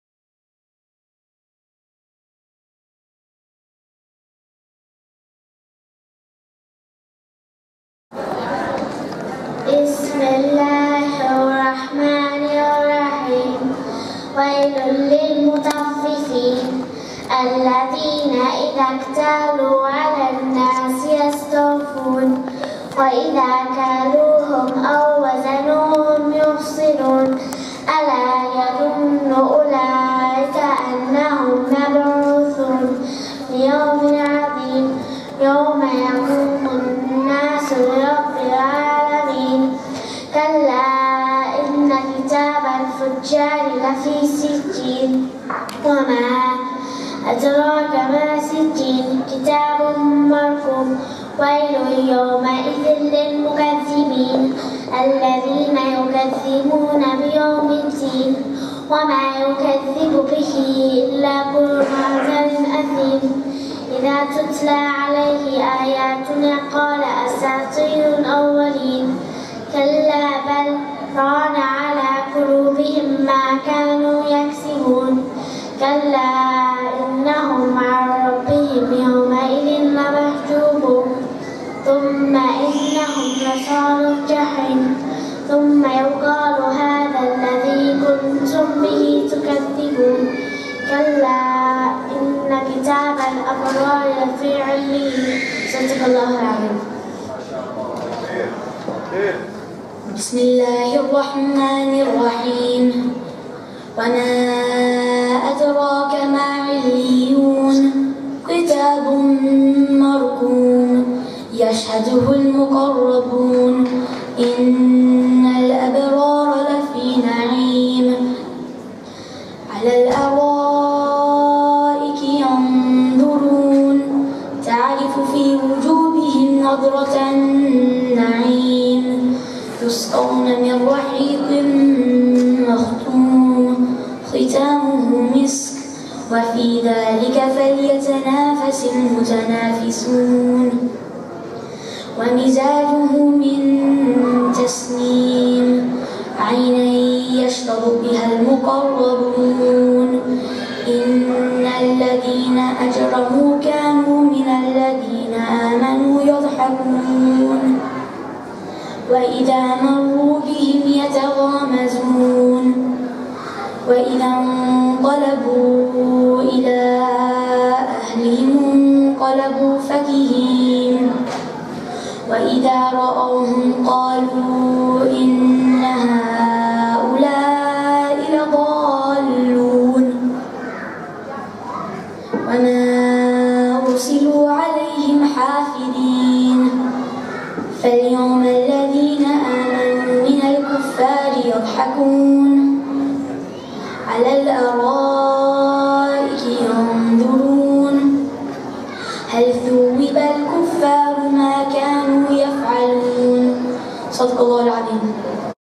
Amina i Salih Mujala / Noć u susret Ramazanu
Islamska Zajednica Bošnjaka u Sydney-u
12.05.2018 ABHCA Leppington
Australia